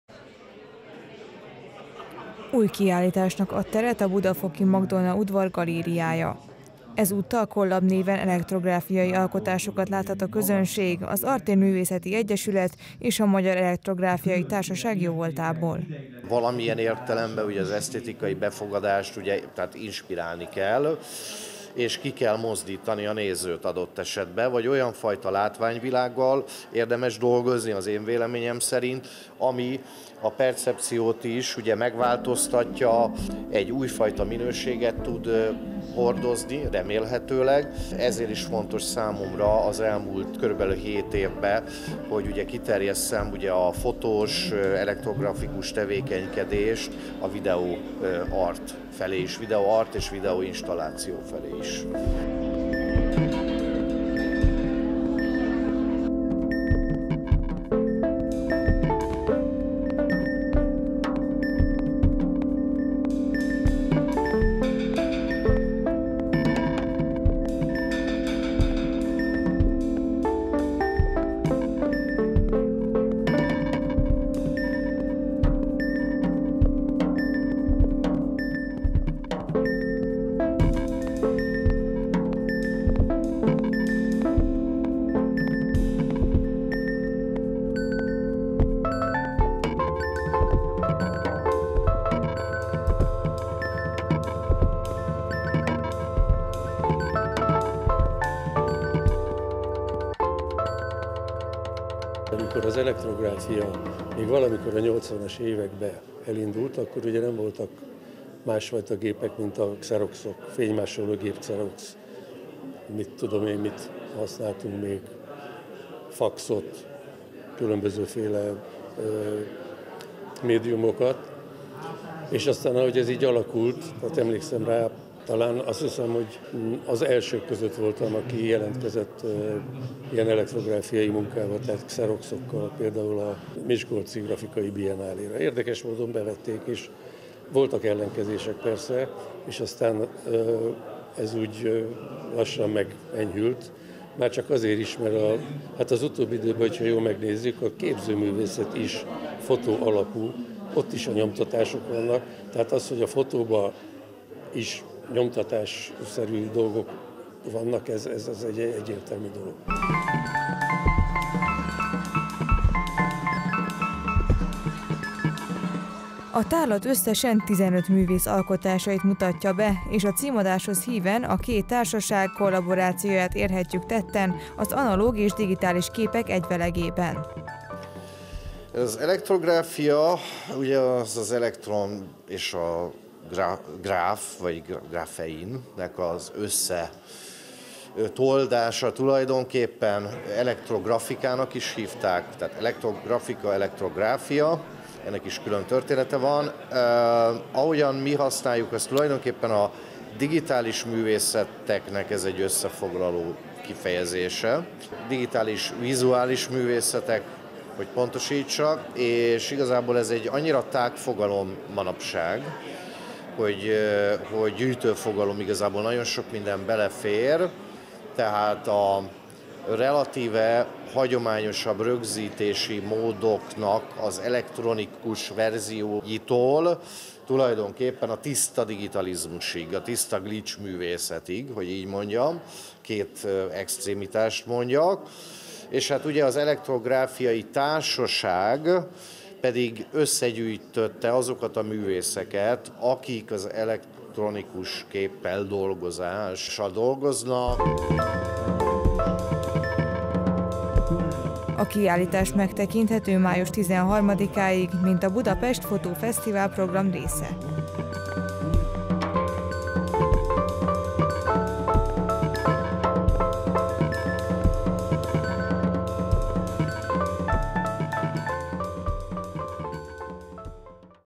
Collab - Elektrográfiai kiállítás a Magdolna Udvarban - 2023.04.19.
Közös kiállítás nyílt az Artér Művészeti Egyesület és a Magyar Elektrográfiai Társaság szervezésében a budafoki Magdolna Udvarban. A "Collab" megnyitóján az elektrográfia mélységeibe pillanthattunk bele.